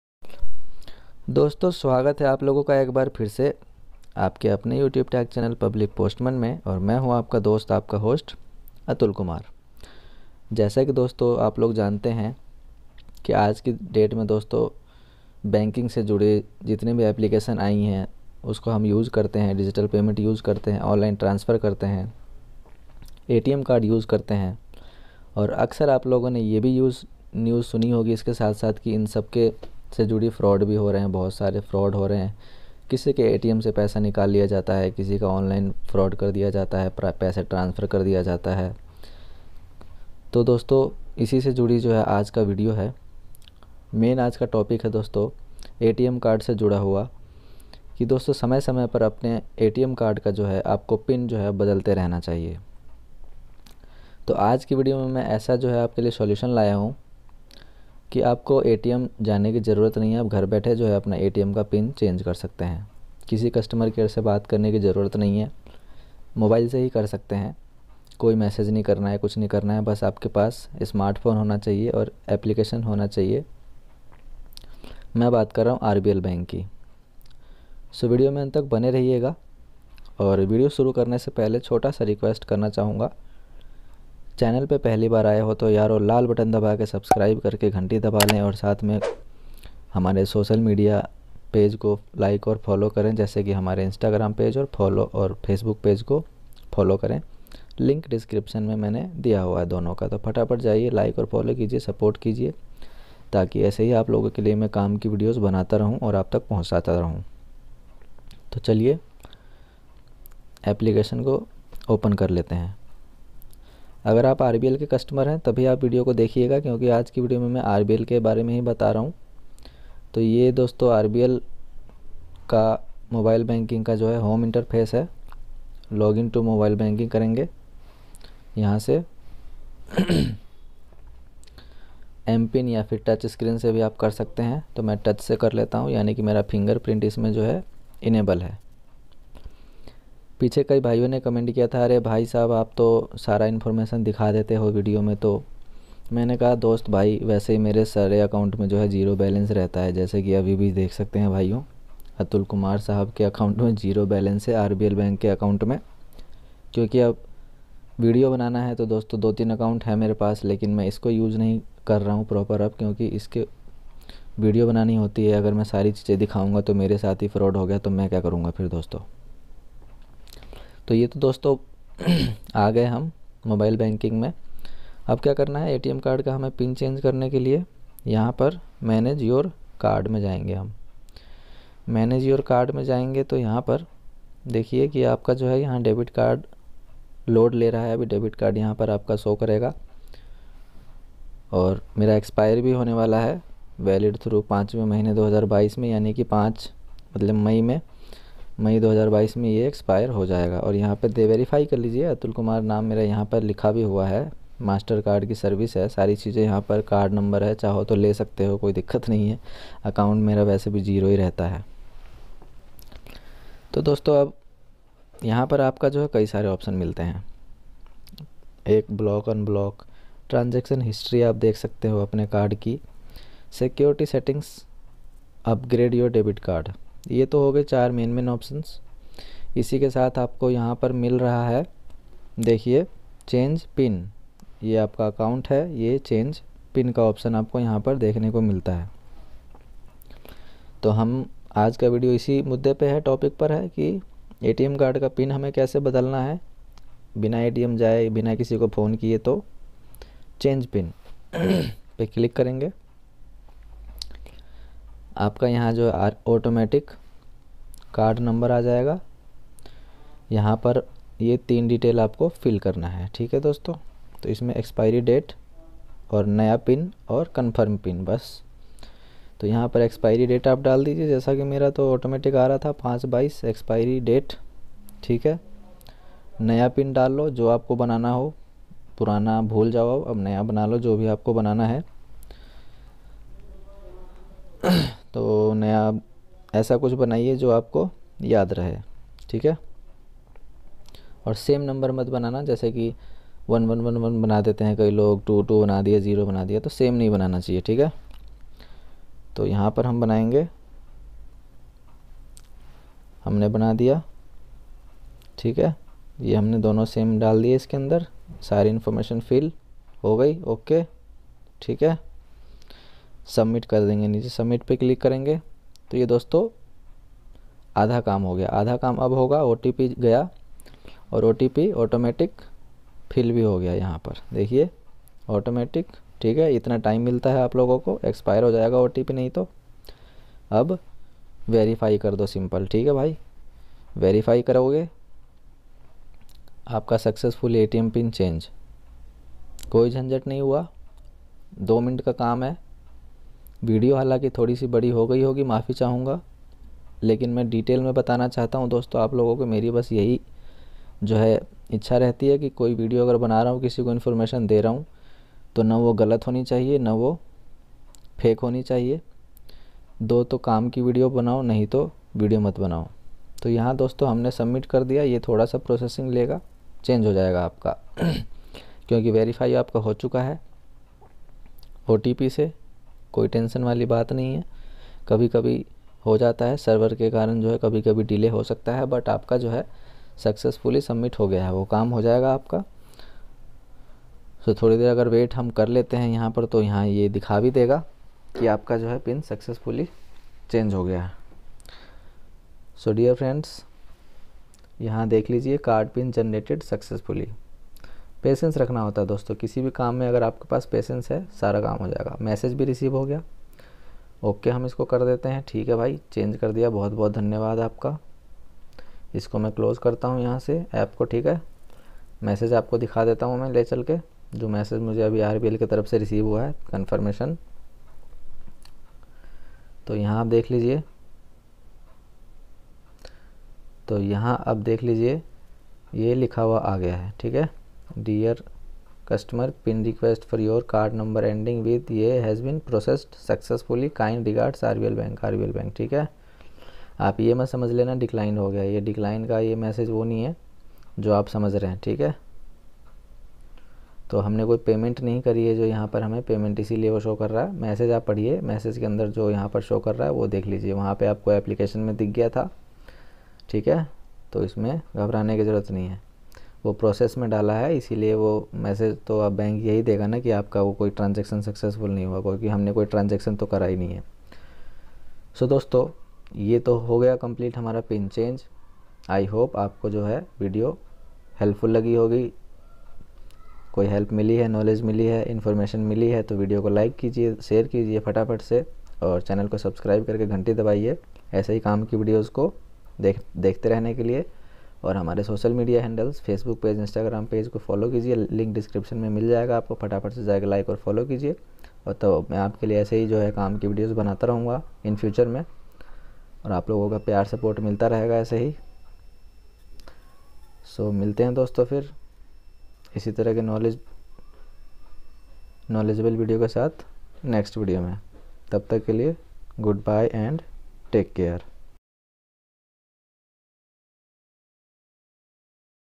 Atm card pin change kaise kare RBL Bank | Rbl mobile banking
About :- Atm card pin change kaise kare RBL Bank | Rbl Bank atm card pin change | Rbl mobile banking

दोस्तों आज के इस वीडियो में मैंने आपको बताया है आरबीएल बैंक के एटीएम कार्ड का पिन कैसे चेंज करते हैं मोबाइल से घर बैठे।
आपको बैंक में या फिर एटीएम में जाने की जरूरत नहीं है अब बिना एटीएम जाए आप घर से एटीएम कार्ड का पिन बदल सकते हैं।
पूरा प्रोसेस सीखने के लिए वीडियो में अब तक बने रहिएगा वीडियो को कंप्लीट जरूर देखिएगा।

और ज्यादा जानकारी के लिए हमारे सोशल मीडिया को फॉलो करें।

HOST :- "ATUL KUMAR"
(Public Postman) @publicpostman